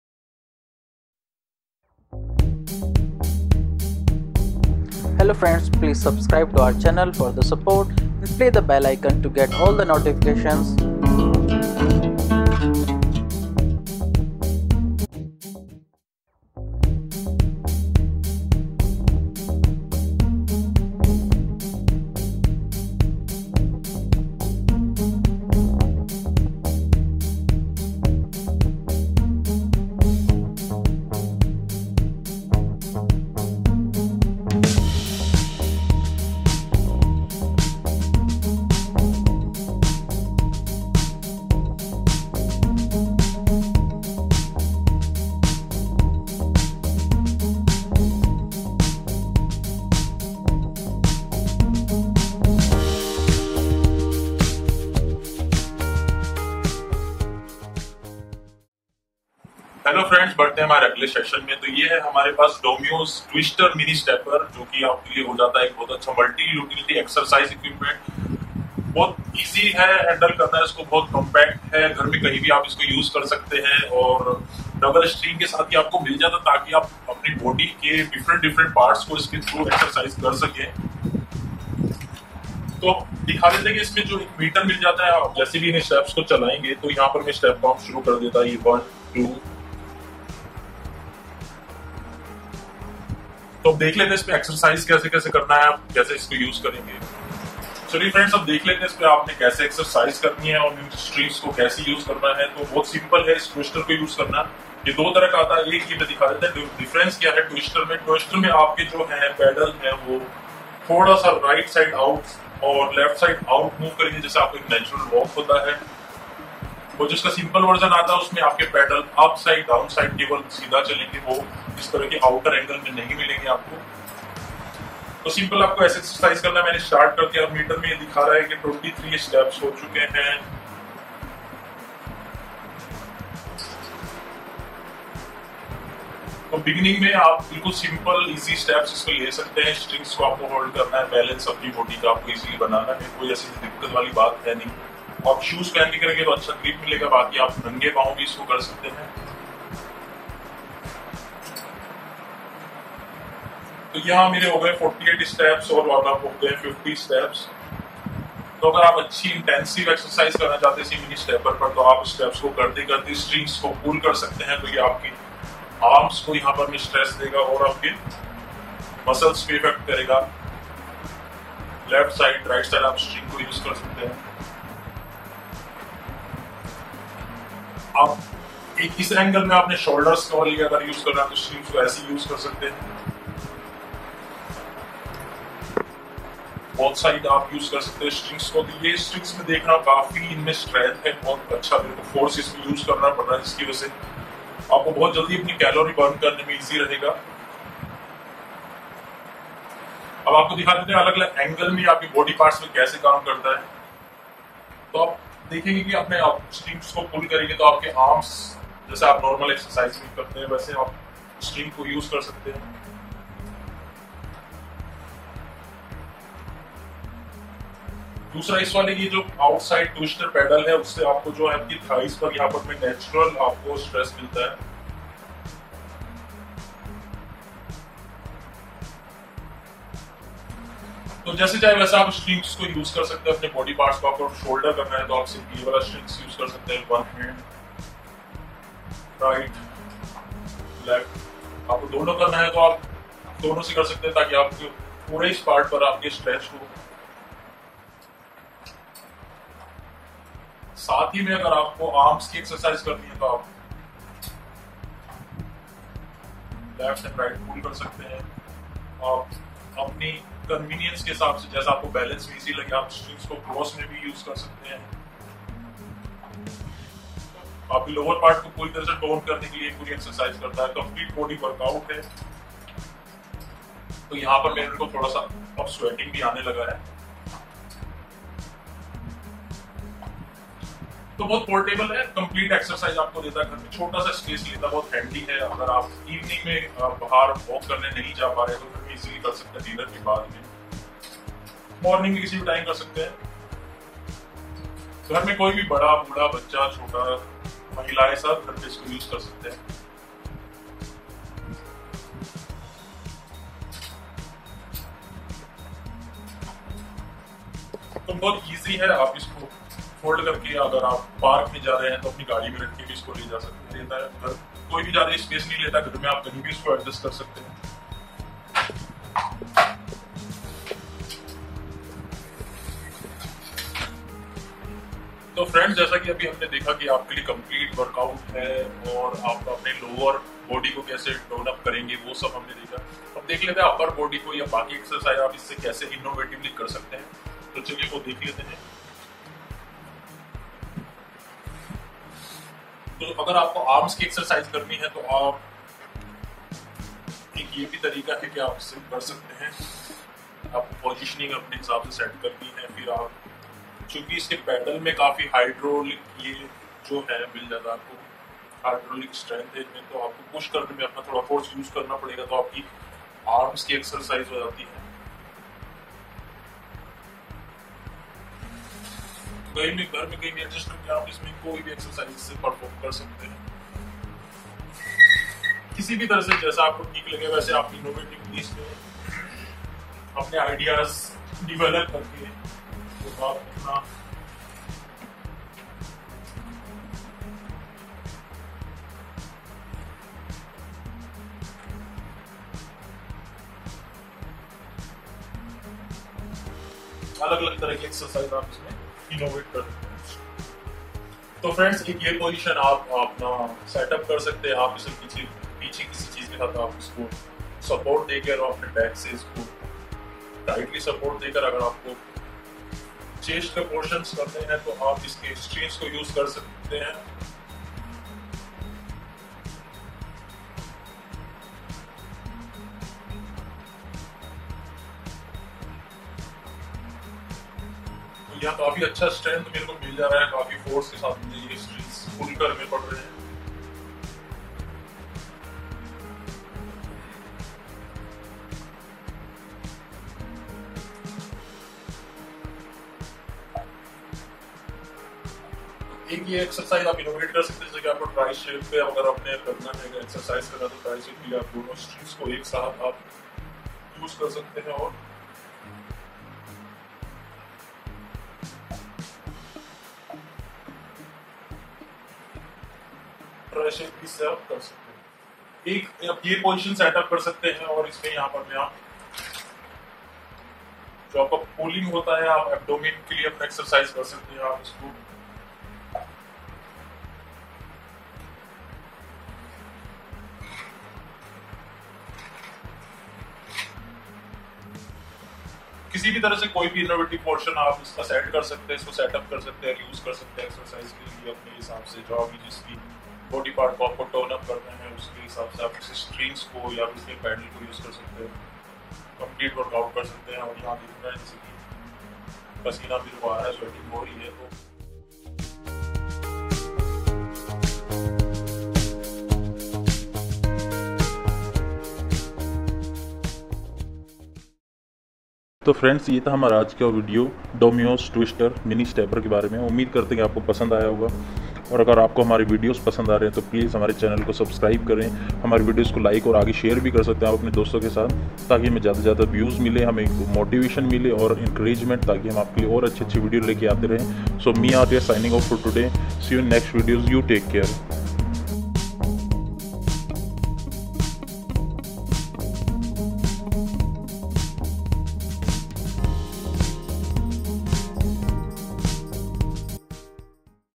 DOMYOS Twister Mini Stepper | सभी व्यायाम के लिए | Reviews in HINDI by Urmil Arya
Dosto es video me hum aapke liye laye hai review of Domyos twister mini stepper ka, jo ki ek complete exercise equipment hai, aur indoor me use kerne ke liue ekdum. adhiya hai...
Ghar me koi phi kabhi bhi aram se es par walk, running, muscles ki exercise ker skate hai. Multipurpose, multifuncttion machine sabhi fitness pasand logo ke liye.
Humne khud use kerke genuine review aur use kerne ki tarike aapko bataye hai. Umeed hai aapko video pasand aye.

Product Features:
VERSATILITY
Improves your cardio fitness and tones your entire body (elastic bands).
PARAMETER MEASUREMENT
4-function display: reps, duration, pace and calorie expenditure.
EASY TO MOVE
Small, compact piece of fitness equipment that's easy to move.
STABILITY
Non-slip steps.

Can be buy from Decathlon store and online.

Our Other Useful and Popular videos are as under